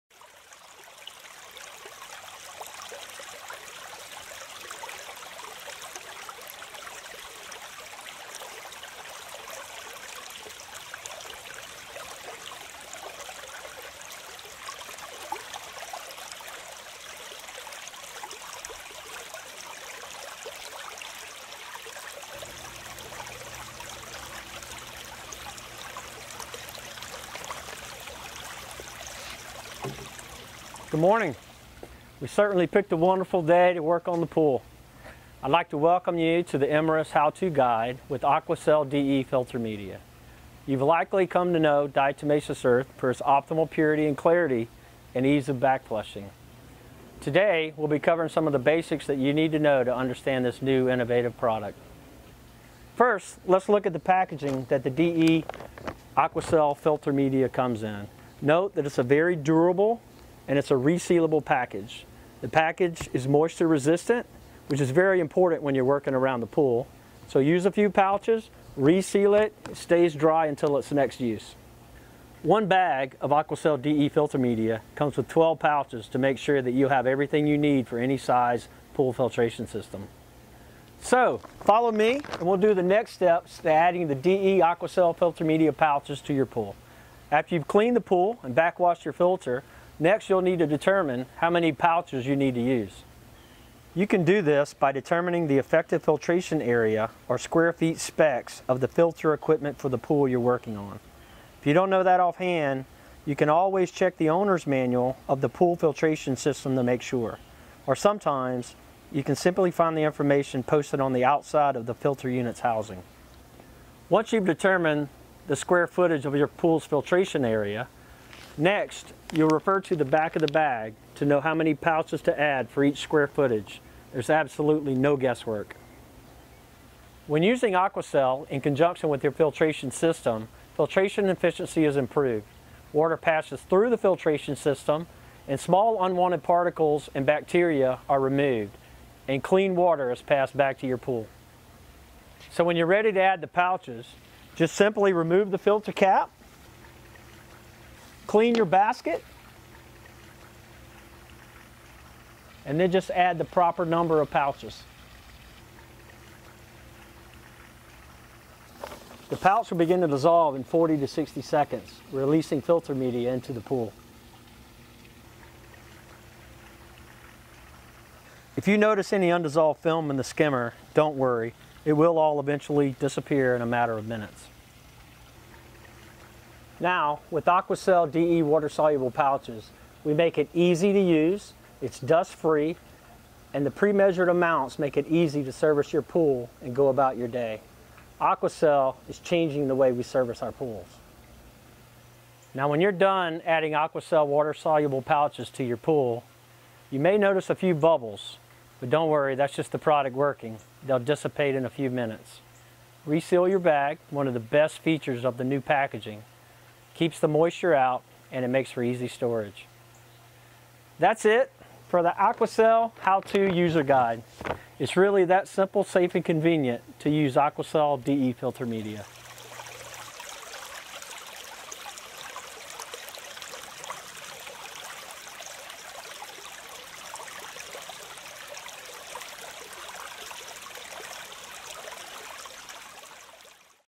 Technology | Aqua-Cel® User Guide - The Perfect Pool Filtration Media | Imerys
Do you want sparkling clean and clear pool water? This video covers what you need to know to understand how to use Aqua-Cel diatomaceous earth filter media, specifically designed for use in swimming pools.